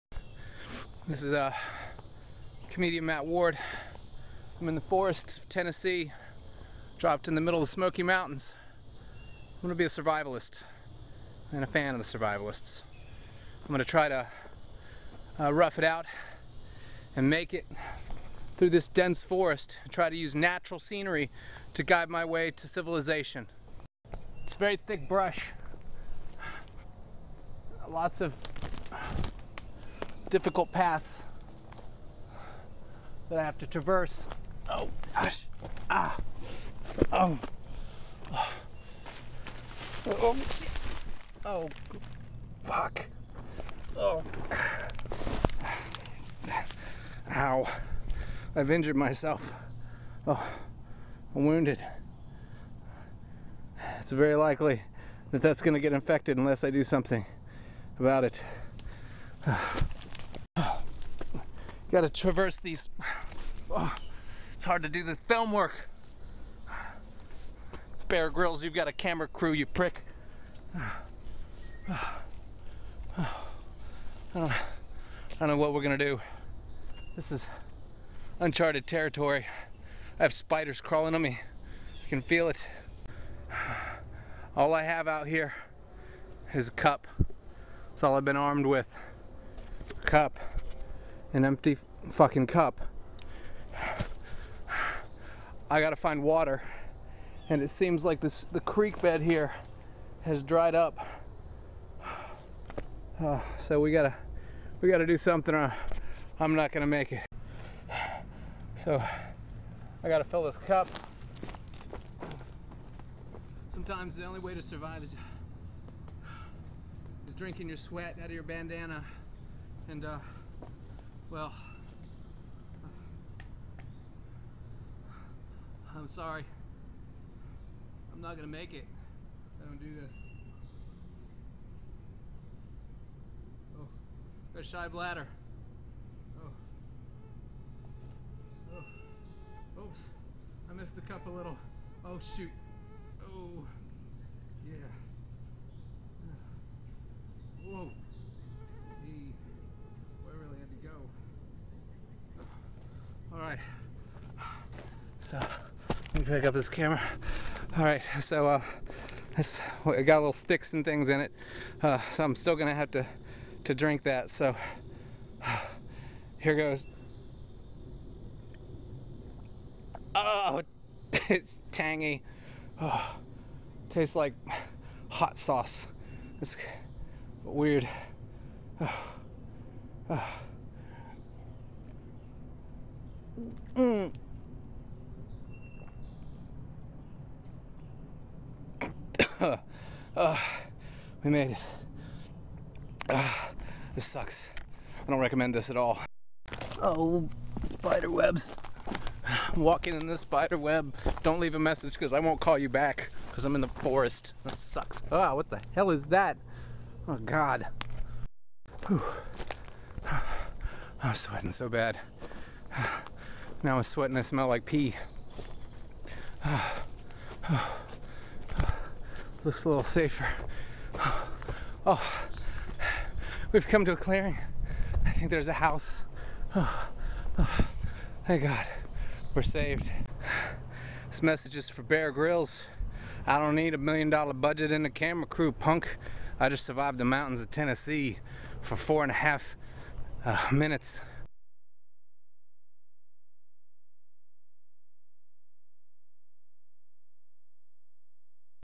Survivorman Parody Matt Ward Smoky Mountains
I am dropped into the middle of the Smoky Mountains where I do my best to use my awesome survival skills.  Check it out!